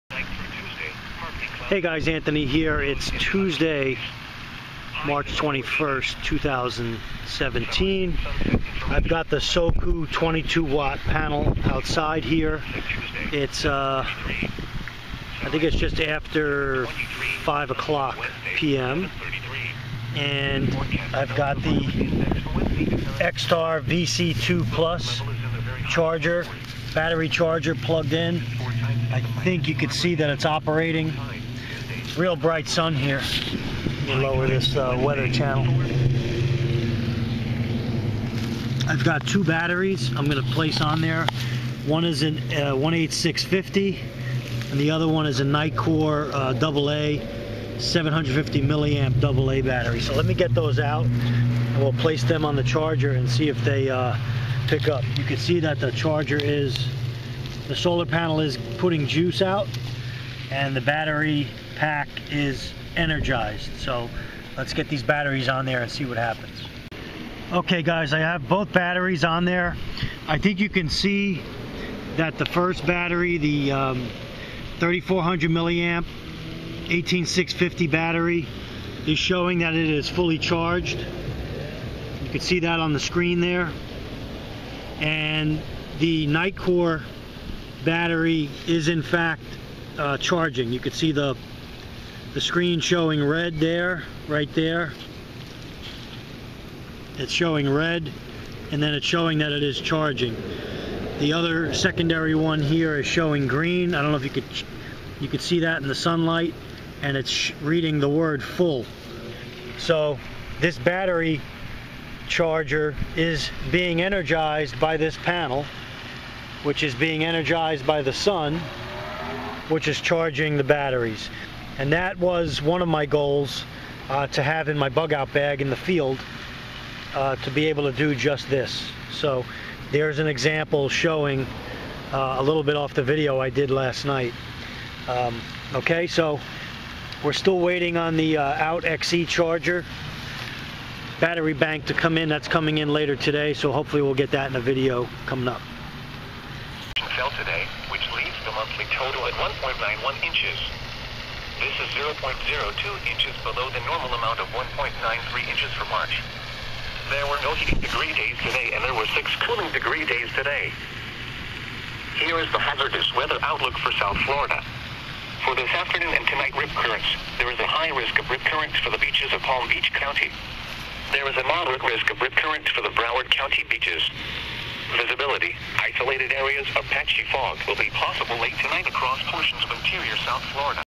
SOKOO 22 Watt Folding Solar Charger and XTAR VC2 Plus Battery Charger- Outdoor Test
In this video I demonstrate how to hook up the XTAR VC2 Plus Battery Charger to the SOKOO 22 Watt Solar Charger in the field.